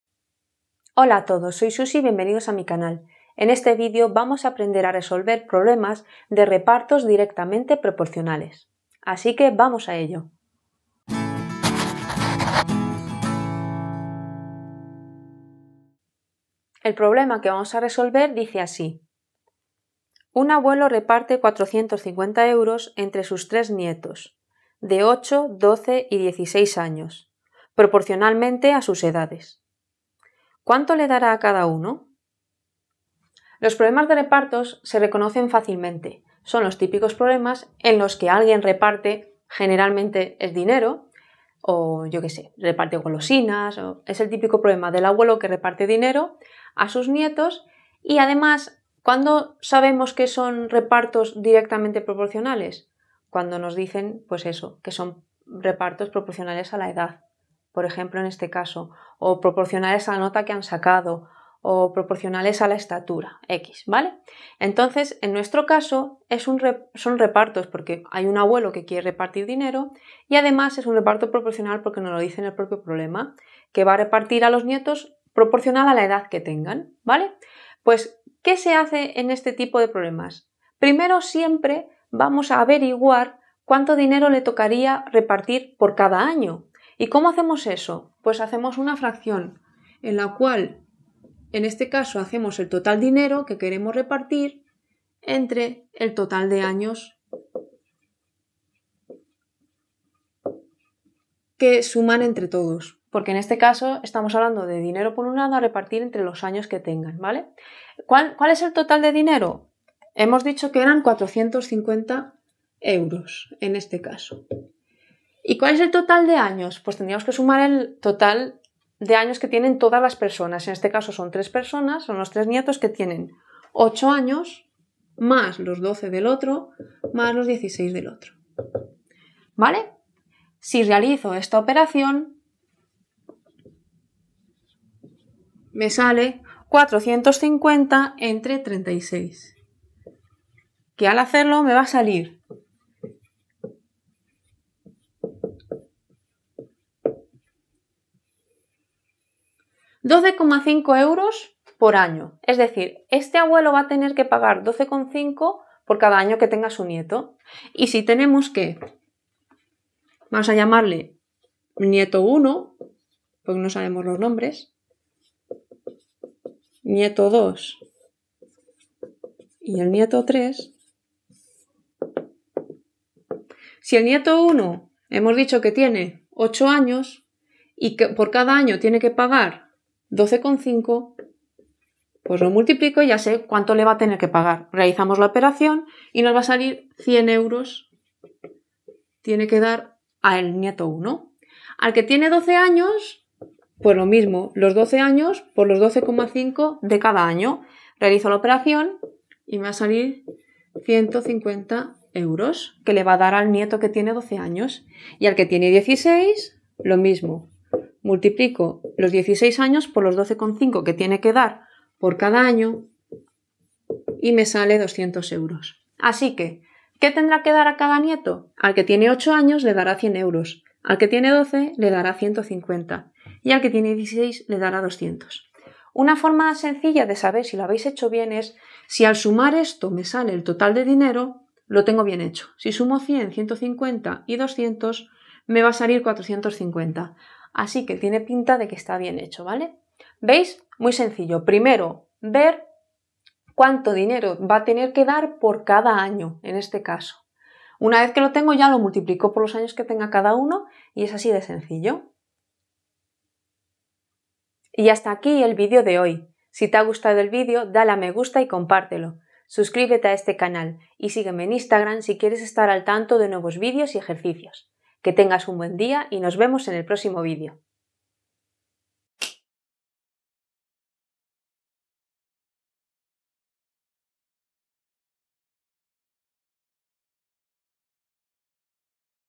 Repartos DIRECTAMENTE Proporcionales ✅ Proporcionalidad
Aprende a resolver problemas en los que hay que hacer repartos directamente proporcionales.

⏰ TIEMPOS:
00:00 Introducción
00:16 Enunciado del problema
00:33 Organizamos los datos
01:20 Cálculo de la cantidad de dinero a repartir por año
02:37 Cálculo del dinero a repartir por persona